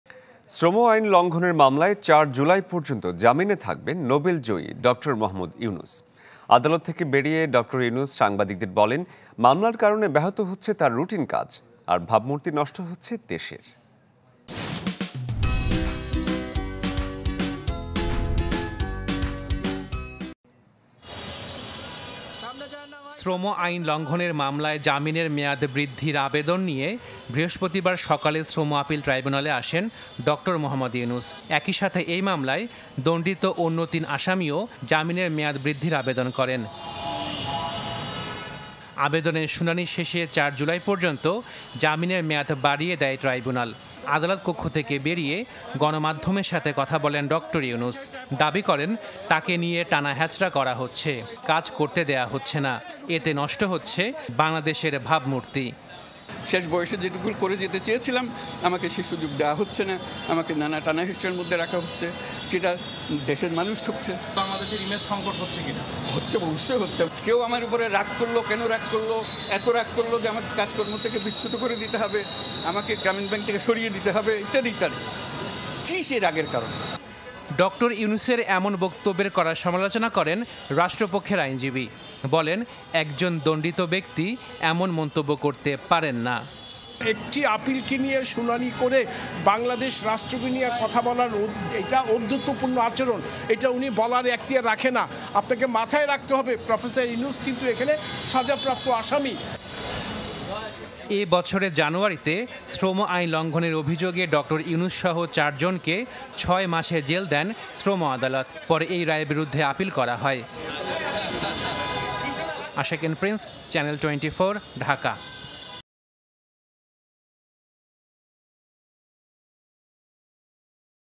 ৪ জুলাই পর্যন্ত জামিনে থাকবেন ড. ইউনূস | Dr. Yunus | Channel 24
৪ জুলাই পর্যন্ত জামিনে থাকবেন ড. ইউনূস | Dr. Yunus | Channel 24
শ্রম আইন লঙ্ঘনের মামলায় ৪ জুলাই পর্যন্ত জামিনে থাকবেন নোবেলজয়ী ড. মুহাম্মদ ইউনূস। পরে সাংবাদিকদের ড. ইউনূস বলেন, মামলার কারণে ব্যহত হচ্ছে রুটিন কাজ। এ পরিস্থিতিতে ভাবমূর্তি নষ্ট হচ্ছে দেশের। PKG.শ্রম আইন লঙ্ঘনের মামলায় ৪ জুলাই পর্যন্ত জামিনে থাকবেন ড. মুহাম্মদ ইউনূসসহ আরো ৩ জন। সকালে শ্রম আপিল ট্রাইব্যুনাল জামিন বৃদ্ধির আবেদন মঞ্জুর করে এই আদেশ দেন।

»» Office Address:
Bangladesh.